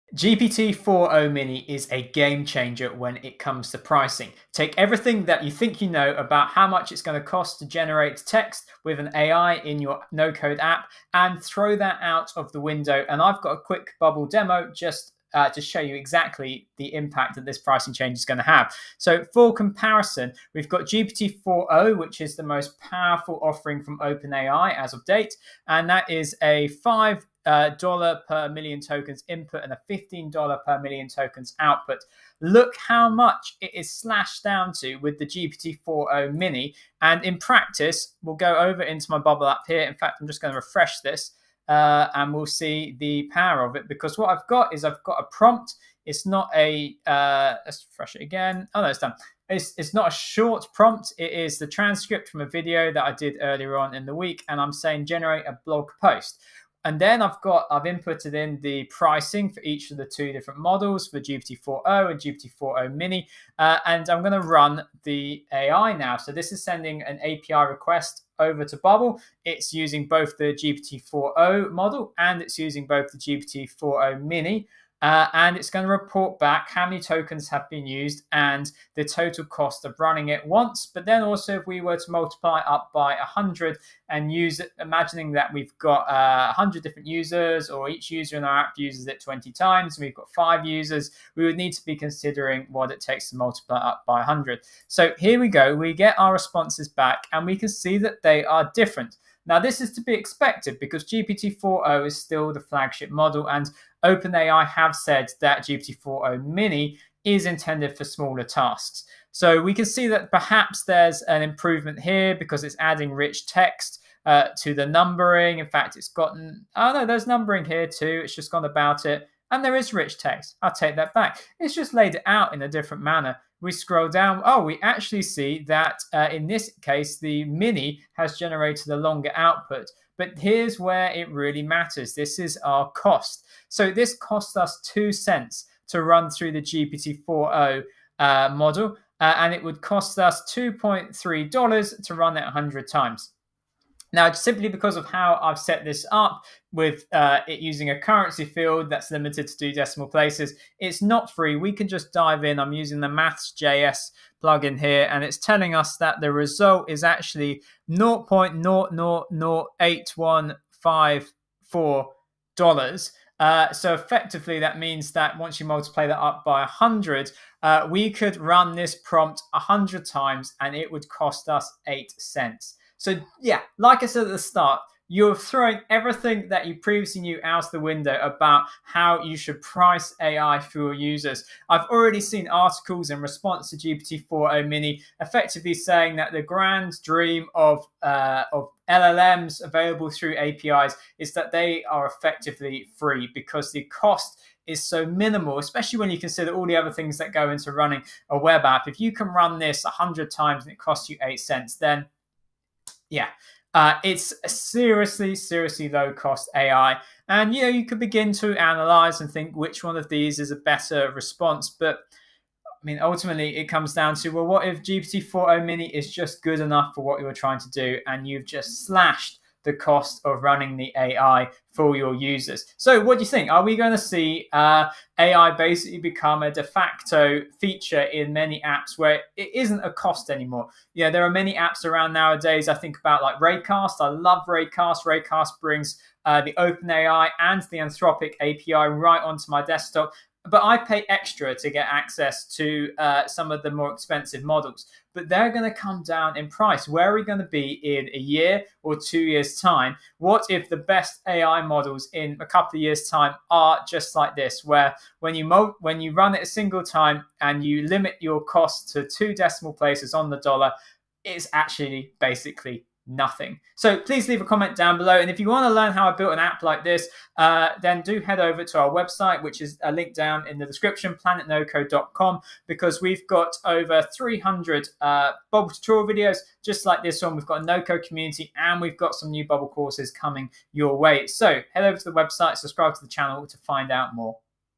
Exploring the Hidden Potential of GPT-4o mini
GPT-4o mini slashes the cost of AI for your no-code SaaS startup. This Bubble.io demo reveals the jaw-dropping price difference between GPT-4 and GPT-4 mini, and how it could make AI a virtually free feature in your app. Discover the potential of GPT-4 mini for your no-code project and how it might shape the future of AI accessibility for solopreneurs and startups.

📈 Accelerate the launch of your startup with our intensive mentorship programme for SaaS founders

00:00 GPT 4.0 Mini: Game-Changing AI Pricing
00:32 Bubble App Demo: Comparing AI Models
00:48 Generating Blog Posts with AI Models
01:09 Cost Comparison: GPT 4.0 vs Mini
01:55 Analyzing AI-Generated Content
02:13 Cost Breakdown: Significant Savings
03:01 The Future of Affordable AI in Apps
03:42 AI as a Standard Feature in Applications
03:55 Evolving AI Pricing and Accessibility
04:25 Learn No-Code AI App Development